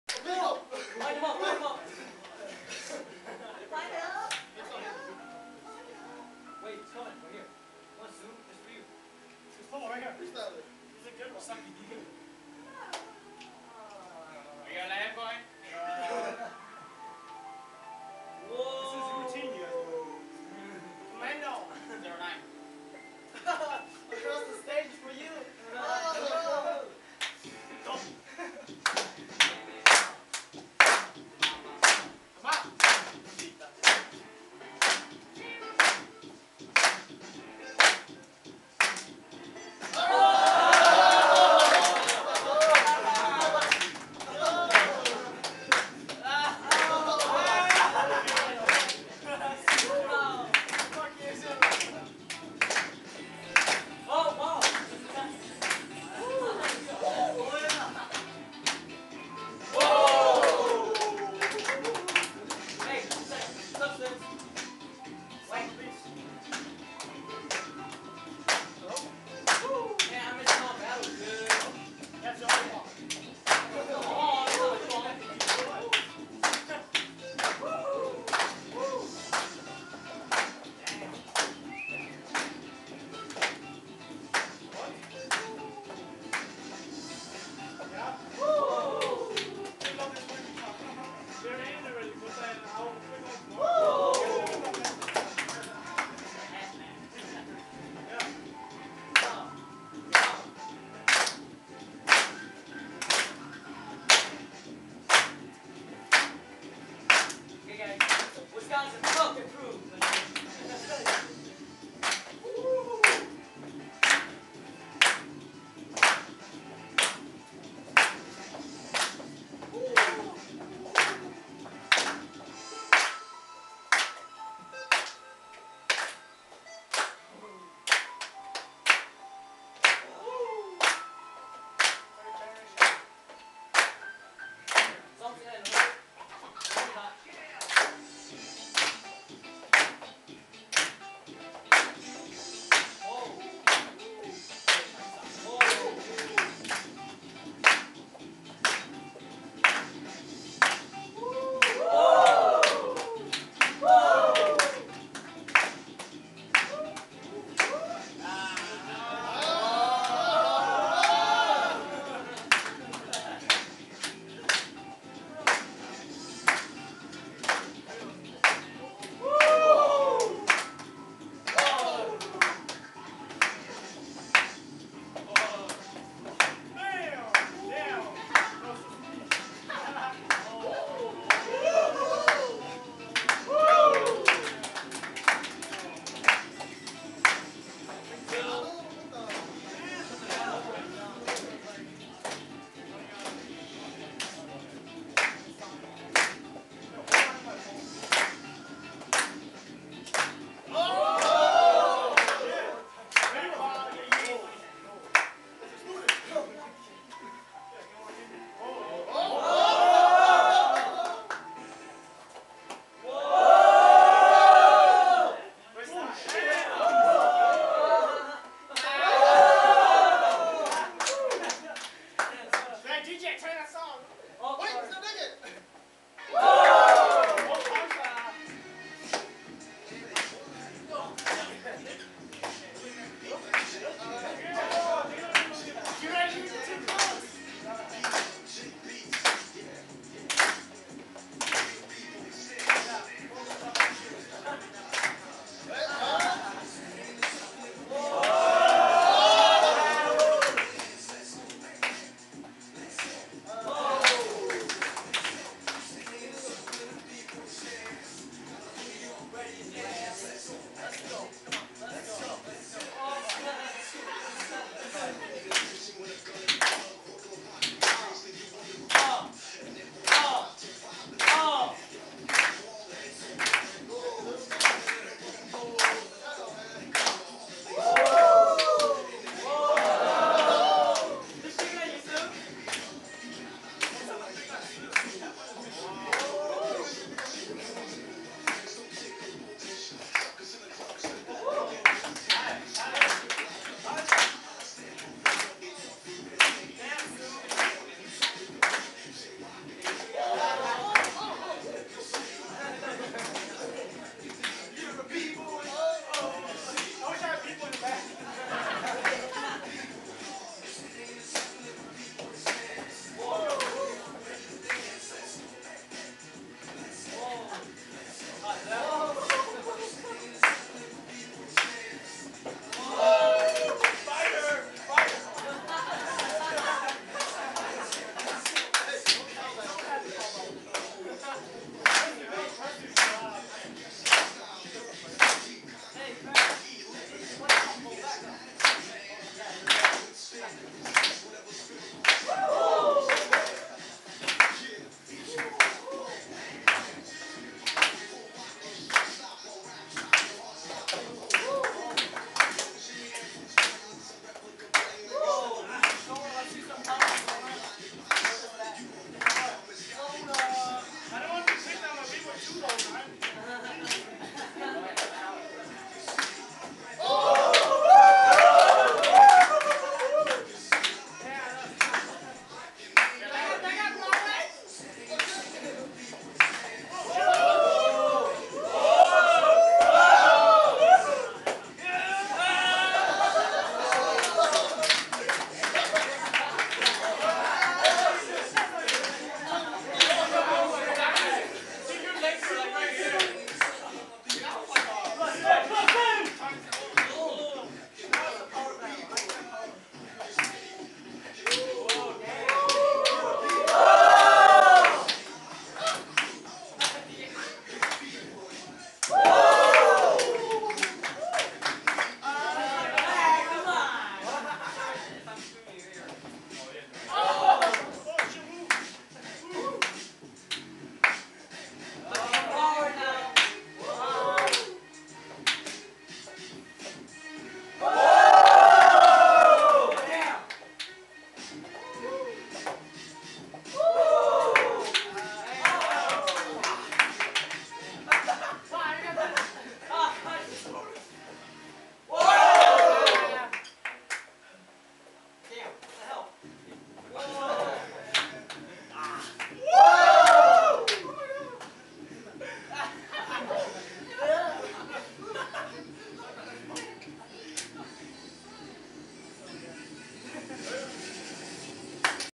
Floor Duster vs Pax-Laws part 4
Right side: Floor Duster (Eau Claire). Left side: Pax-Laws (Wausau). sorry but there's 9 parts of this vid to finish it. Please decide who you think won the battle.